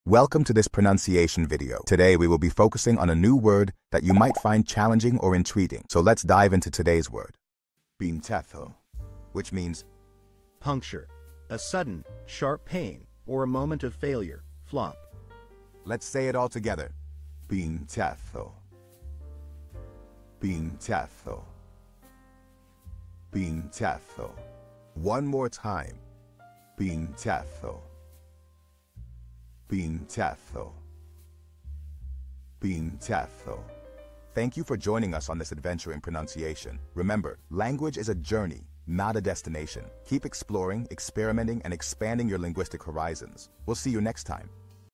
How to pronounce Pinchazo
Master the Pronunciation of 'Pinchazo Which Means Punchazo' - which means : Puncture;Sudden, Sharp Pain;Or a Moment of Failure, flop. 📝 with @PronunciationSpanish 🎓🗣️ - [Your Guide to Spanish]

Learn to pronounce 'Pinchazo' perfectly in Spanish with our easy and engaging tutorial by @PronunciationSpanish! 📖📽️ In this comprehensive video, we provide you with native speaker examples 🗣️👂 and phonetic breakdowns 🎵 to ensure you get the pronunciation just right.

How to say Prick in Spanish